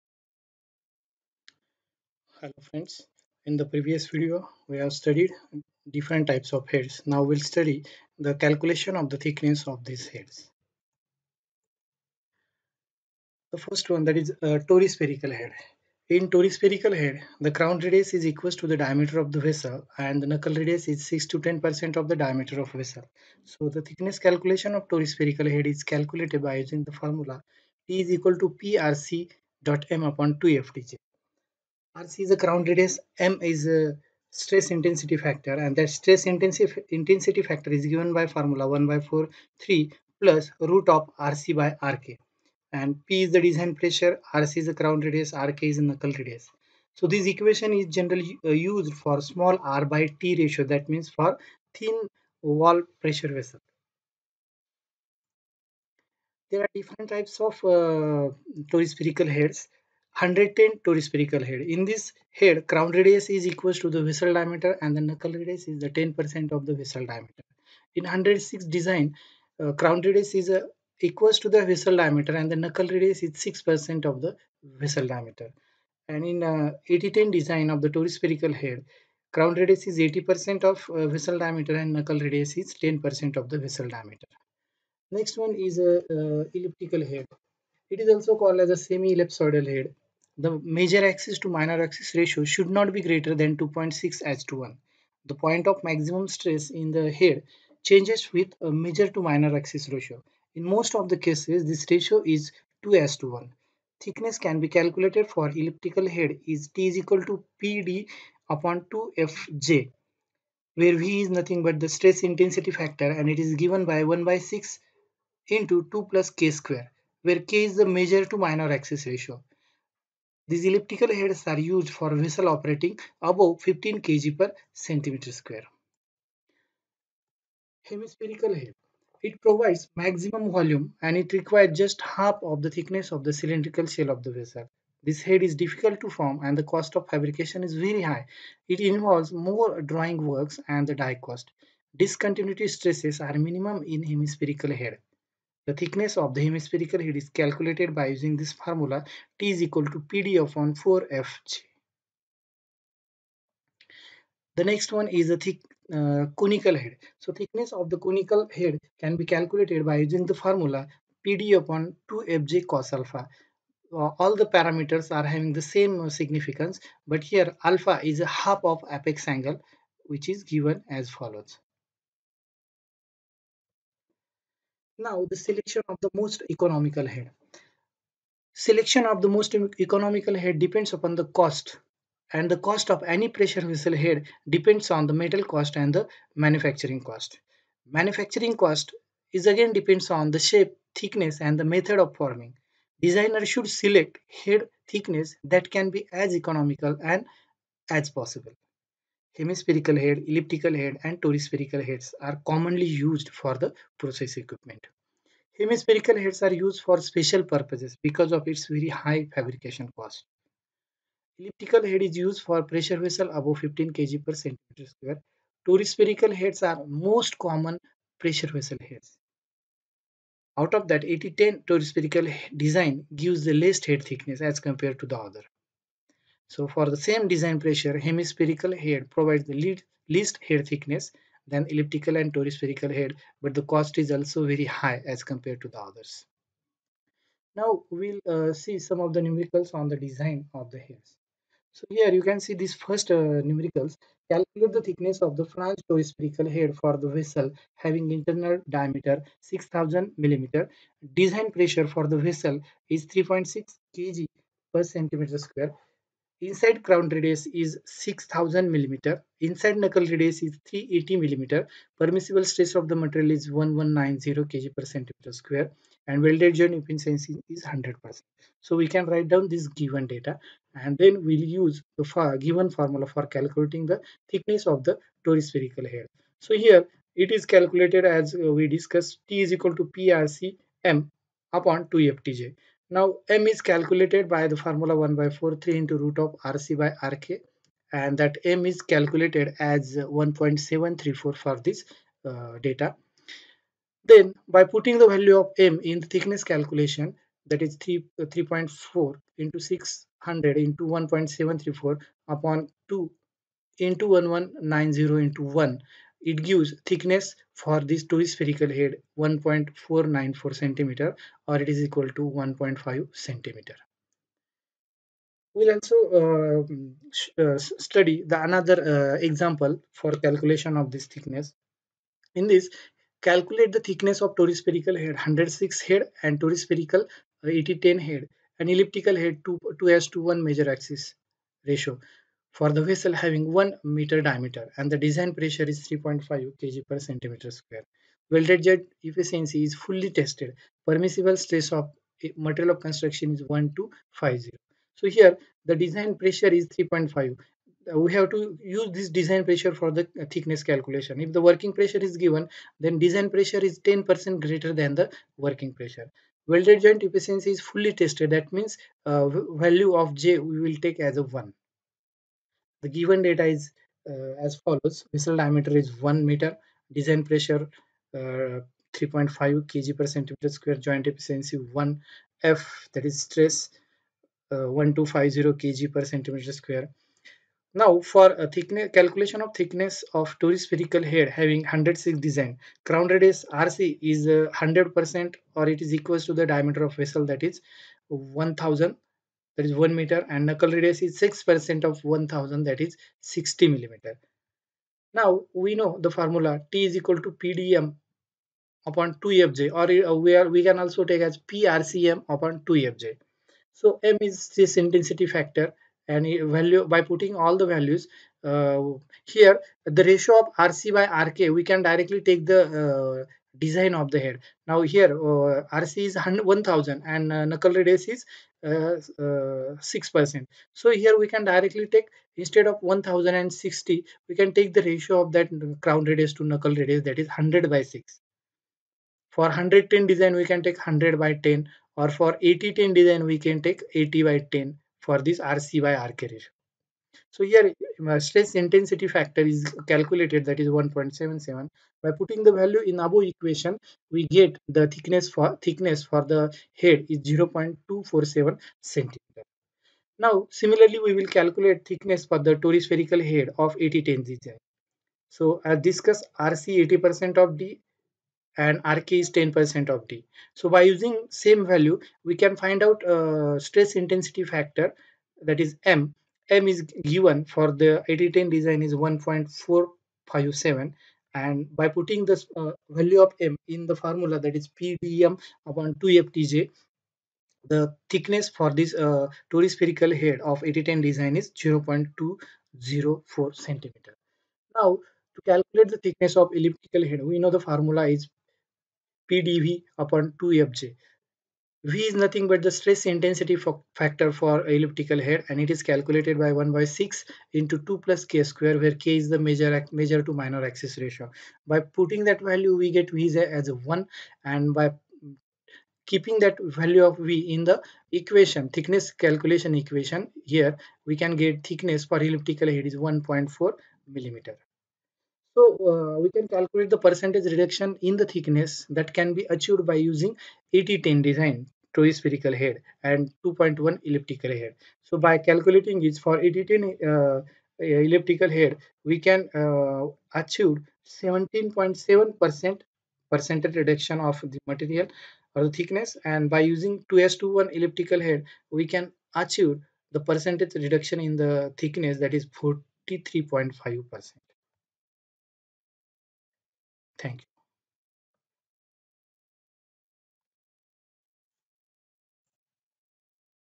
Thickness Calculation of pressure vessel heads
This video helps for solving head thickness calculation problems.